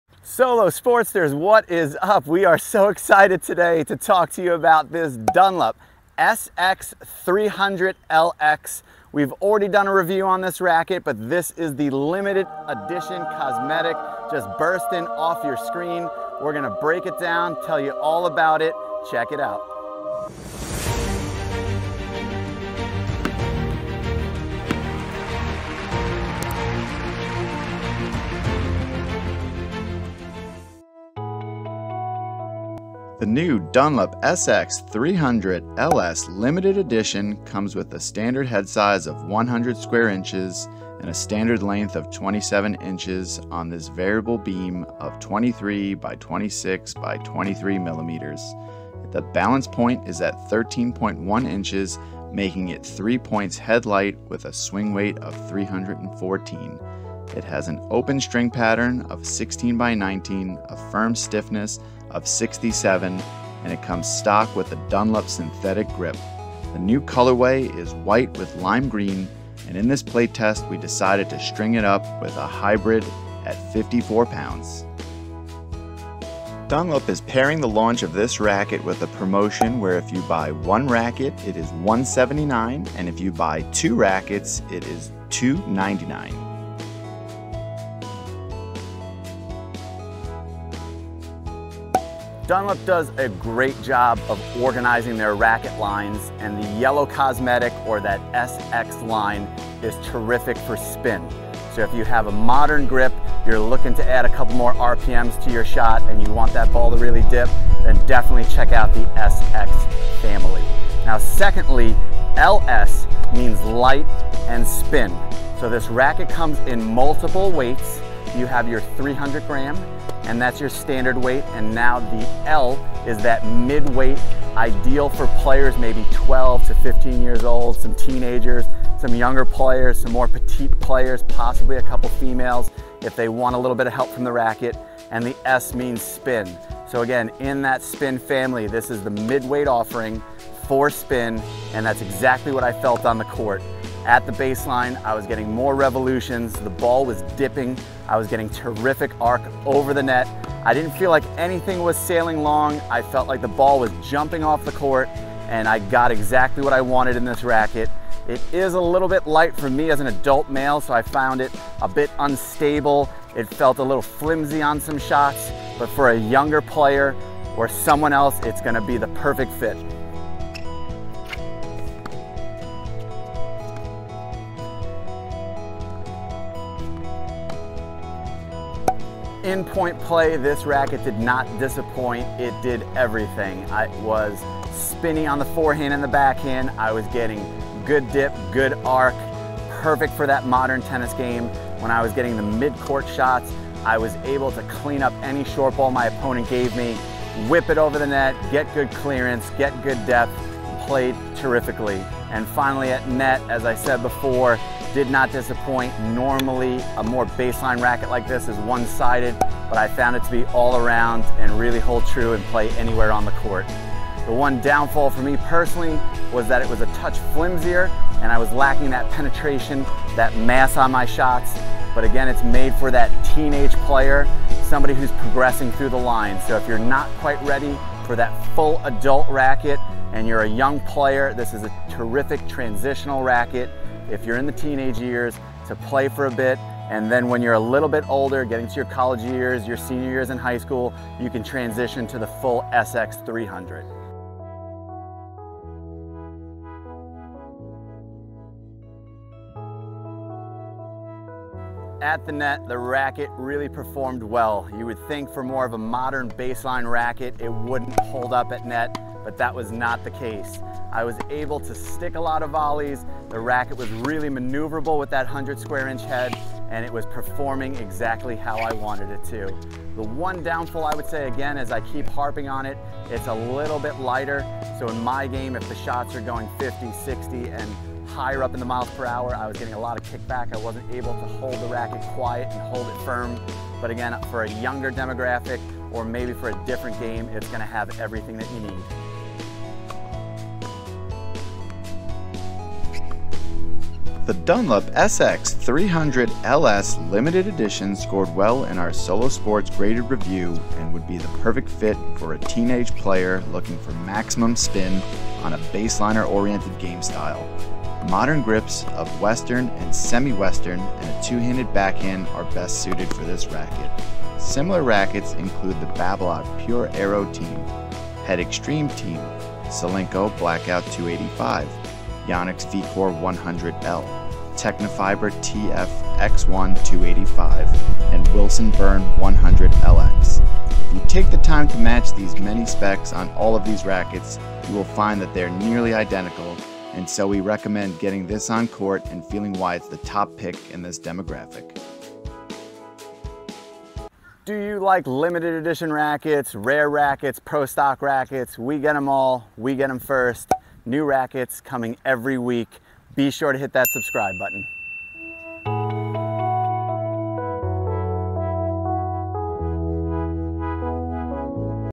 Dunlop SX 300LS Limited Edition Tennis Racket Review
Dunlop SX 300LS LE tennis racquet review by Solow Sports.

This limited edition lighter version of the SX 300 provides very good maneuverability and easy handling. Perfect for advanced players and performance players who are looking for a racket with a lighter weight to achieve fast hits with a high spin level.

Headsize: 100 sq. in.
Length: 27 in.
Weight (strung): 10.6 oz.
Stiffness: 67
Balance: 13.1 (3 Points Head Light)
Cross Section: 23/26/23mm
Swingweight: 314
String Pattern: 16x19
Grip: Dunlop Synthetic